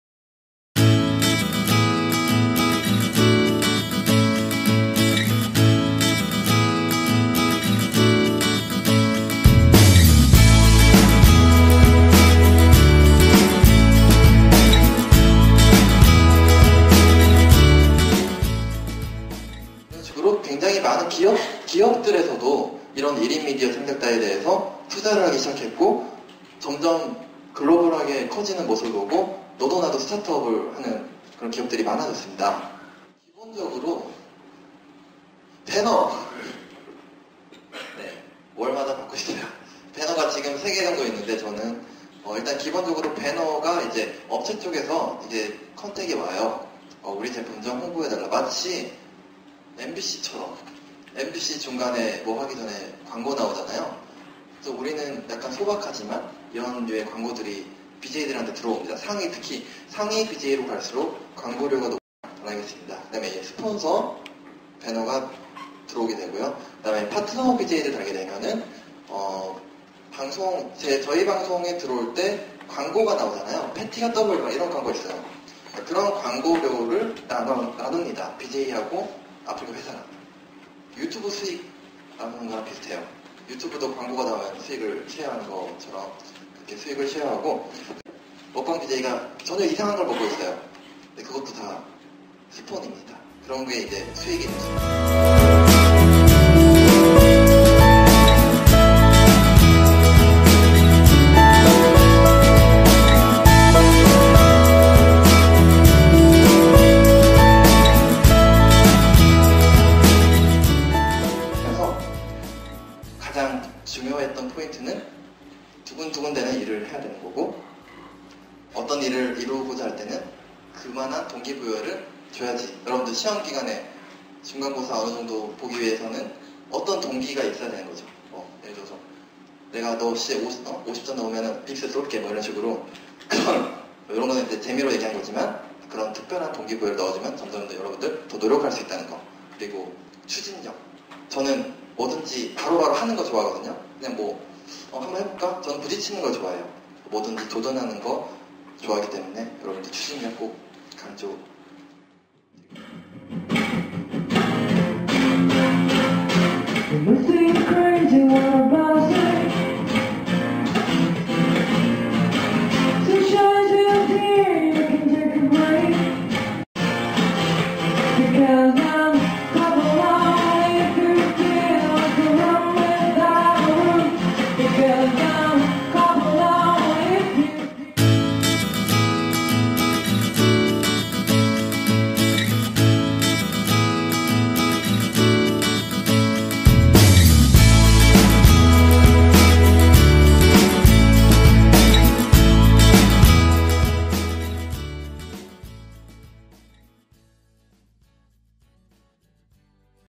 제4회 청춘불고_"프로게이머 이윤열의 1인미디어 제작자 도전기"
청춘들의 고민을 해결하고 취업을 위한 강좌
전 프로게이머 이윤열 선수 후기 동영상입니다.